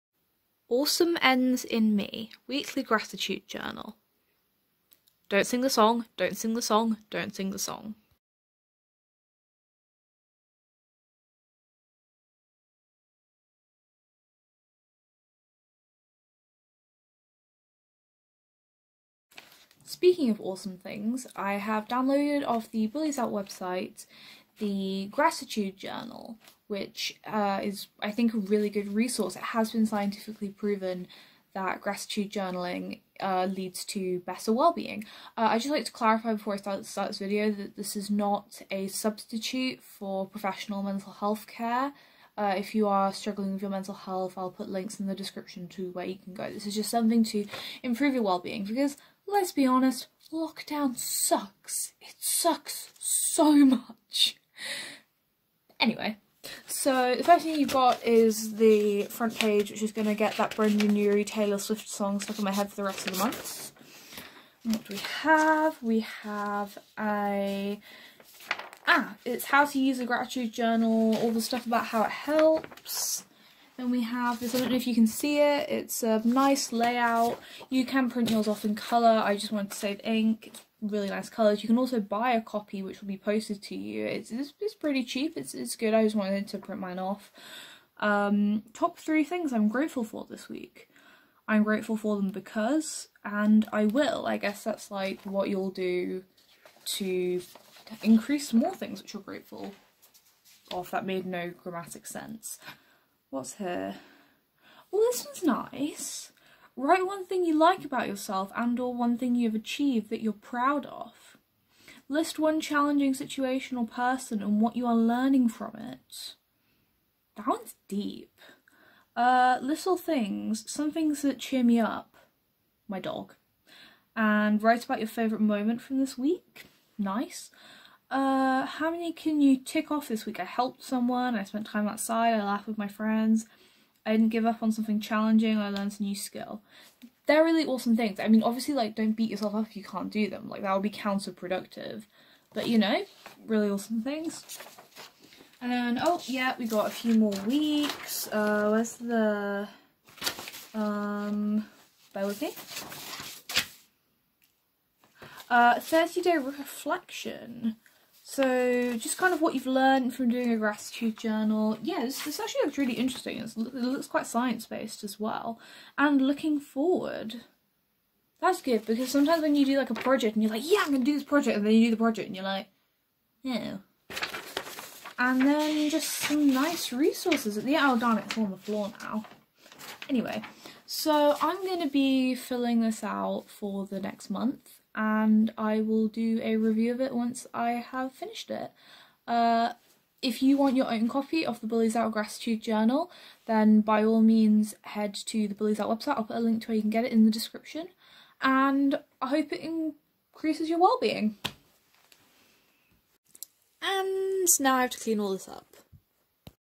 BulliesOut Gratitude Journal Unboxing (kind of) + free gratitude journal to print :)
Hiya! I hope you enjoyed this video. To get your own copy of the BulliesOut gratitude journal posted to your door go and remember you can print a copy for free.

I'd just like to clarify that although gratitude journaling increasing wellbeing, this is not a replacement for professional mental health support. If you are struggling with your mental health, please call Samaritans on 116 123 and seek professional help.